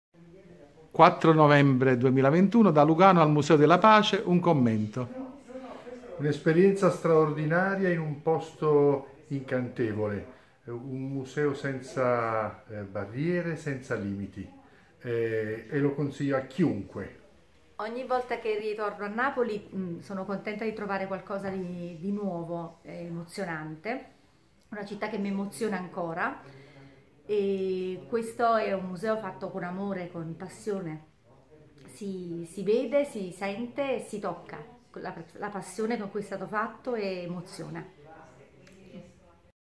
ANITA FLORIO IN VISITA AL MUSEO DELLA PACE - MAMT
La dottoressa Anita Florio - dirigente della Regione Campania dell’Unità operativa “Promozione e valorizzazione Musei e Biblioteche” - ha visitato nuovamente il Museo e la sua Biblioteca, accompagnata dal marito, dalla sorella e dal cognato.
In questa occasione è stato espresso apprezzamento per la continua evoluzione del Museo e per la realizzazione dei percorsi dedicati agli ipovedenti con l’installazione di targhe braille e planimetrie tattili.